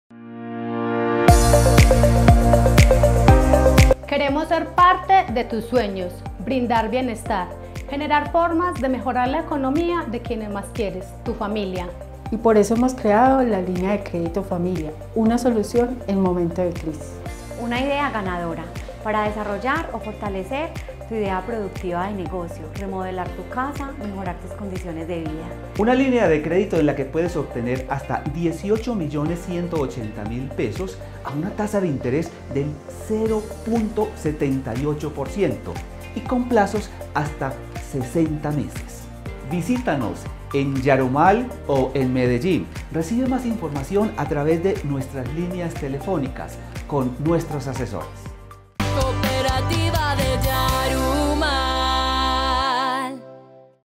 Crédito Familia
Celebración día de padres.
Solución en momentos de crisis.
Una propuesta ganadora para todos los asociados, para que reactiven su economía
126 bonos de $100.000 para la apertura de cuenta (padres o madres que deseen asociarse)
Tasa de interés del 0.78% para mejorar las condiciones económicas de todos.
800 regalos para padres y madres mayores de 25 años (con previa inscripción en la página web)
Es nuestra apuesta cooperativa para exaltar el sentido de la solidaridad y la cooperación en momentos de crisis.
Aplica condiciones y restricciones.